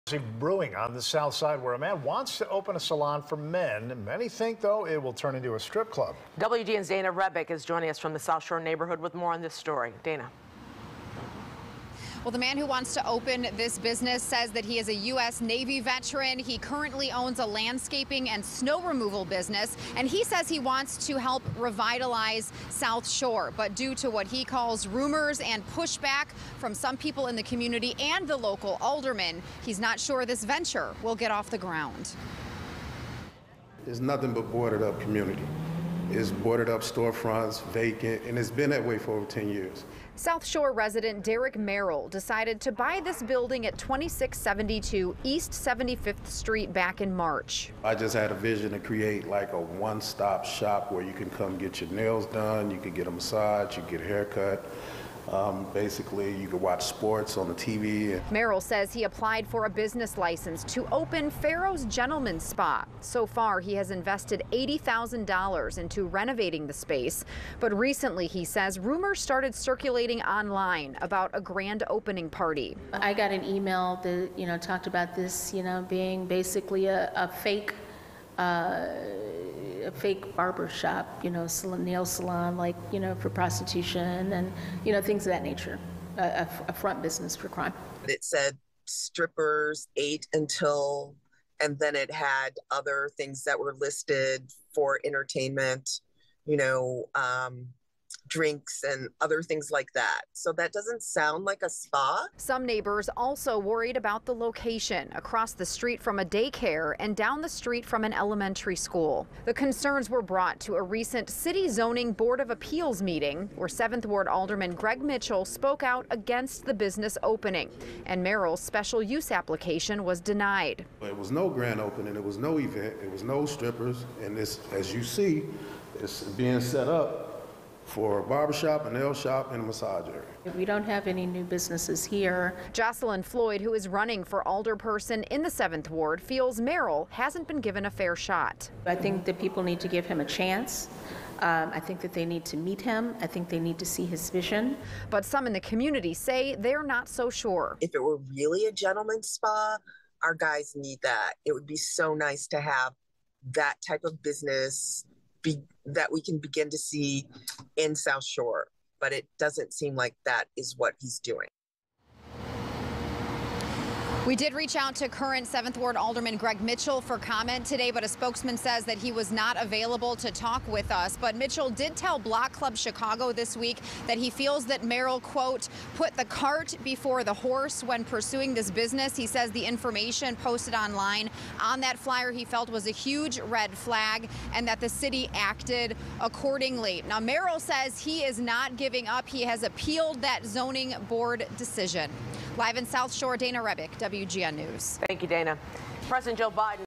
Man fighting to open South Shore gentlemen's spa after rumors of strippers
A man trying to open up a spa in South Shore is fighting to open following rumors.